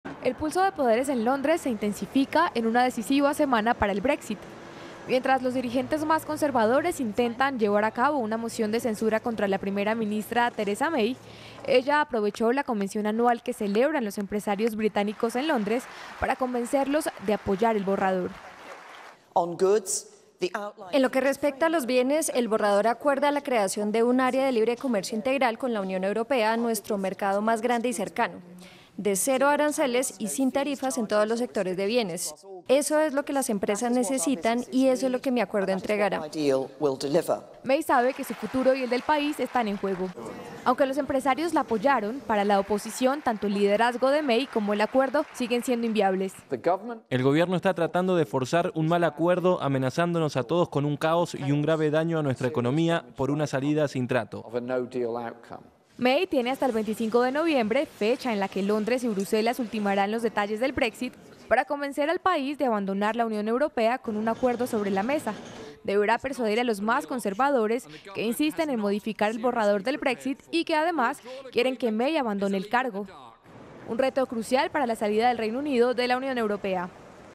Inició una semana crucial para Theresa May
Durante su intervención en la conferencia anual de la Confederación de la Industria Británica, que se llevó a cabo en territorio londinense, May aseguró que dicho acuerdo para la salida de la Unión Europea está completamente pactado y que, debido a eso, se dificultará poder hacer los cambios pedidos tanto por conservadores y como por opositores.